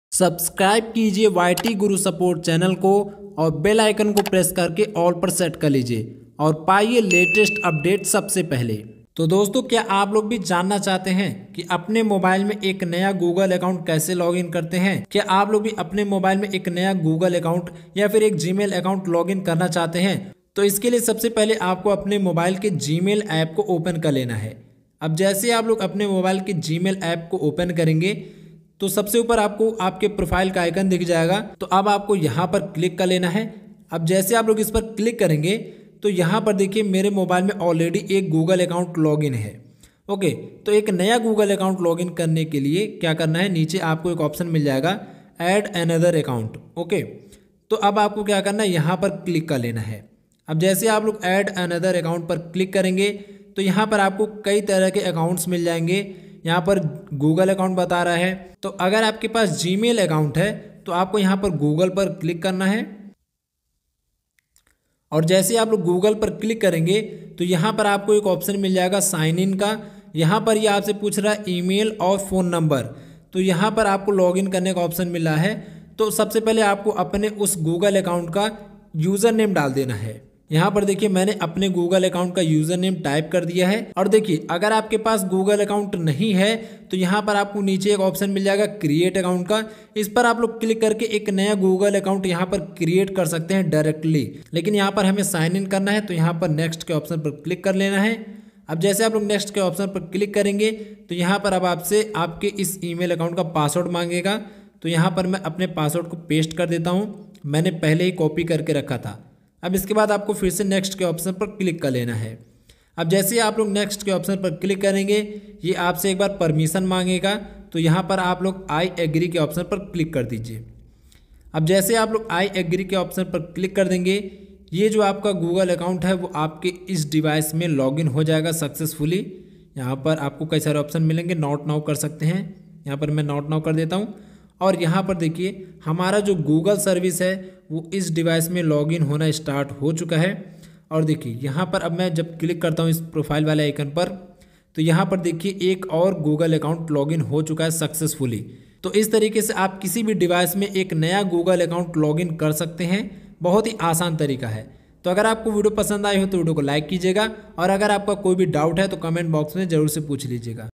Google account kaise add kare (in Android) | How to add google account in mobile
Dosto ish video me maine bataya hai ki, Android mobile me Google account kaise add kare.